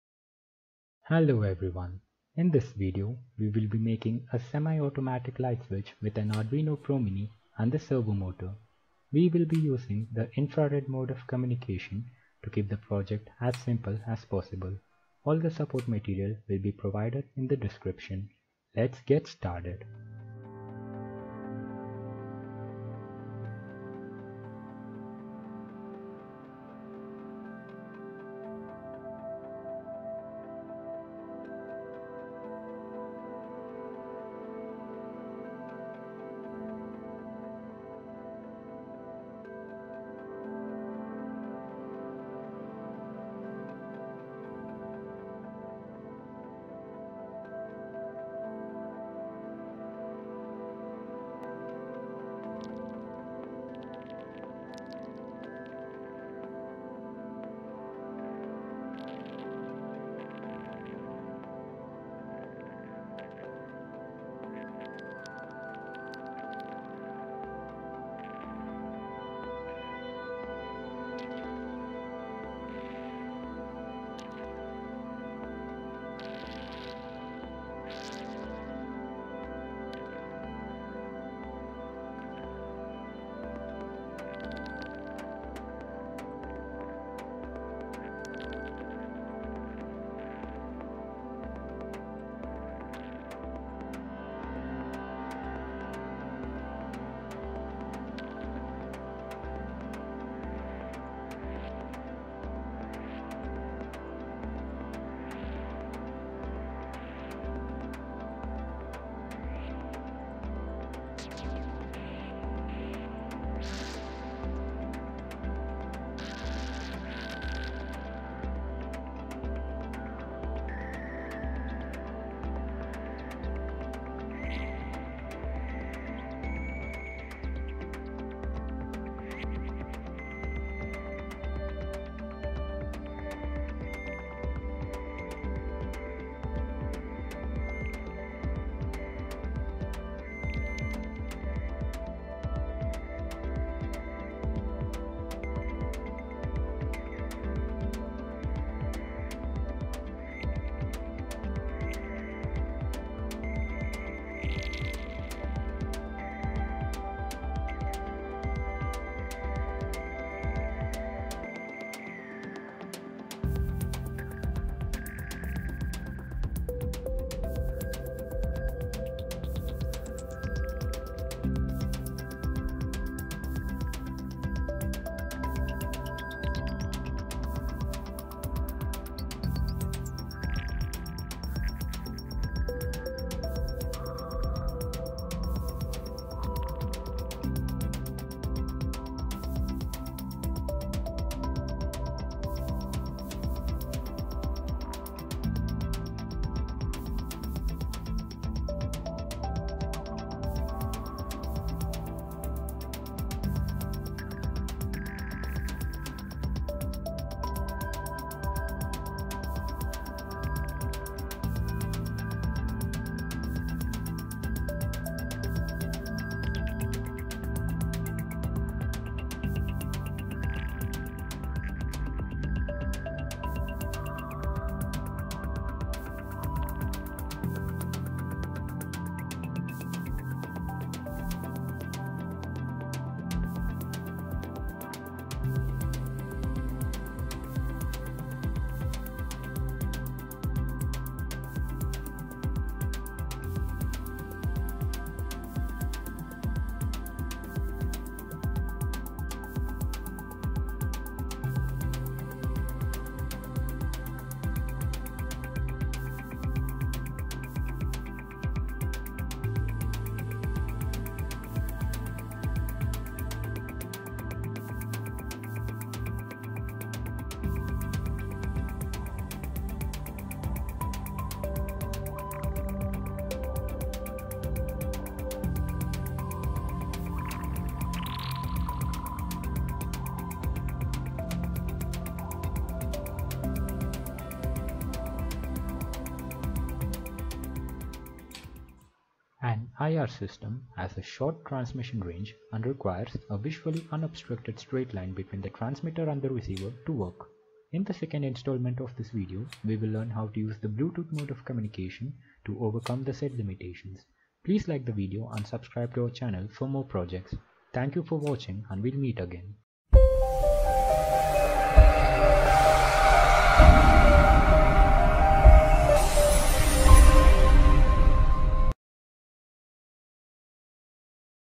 MAKE A SEMI-AUTOMATIC LIGHT SWITCH USING AN ARDUINO PRO MINI AND A SERVO MOTOR
This video will guide you through the process of setting up an Infrared based light switch operated by a servo motor using an Arduino ProMini.